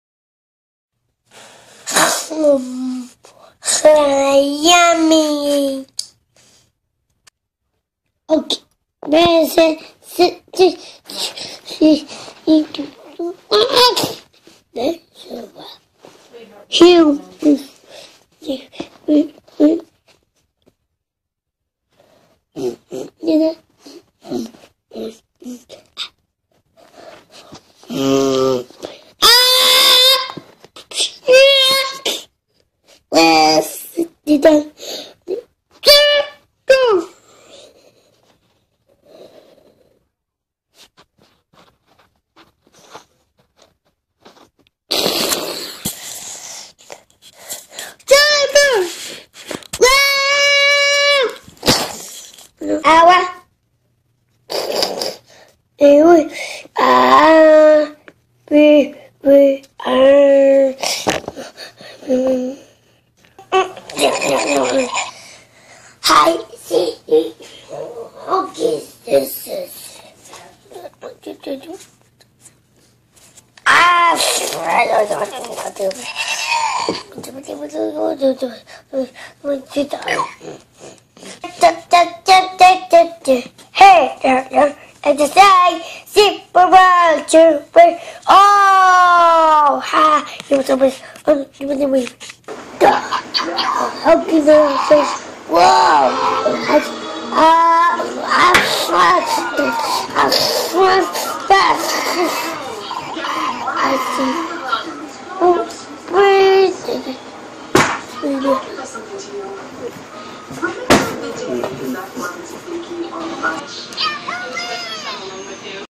Eddie Playing Video - 040914
6 year old with autism recording himself playing with some of his favorite toys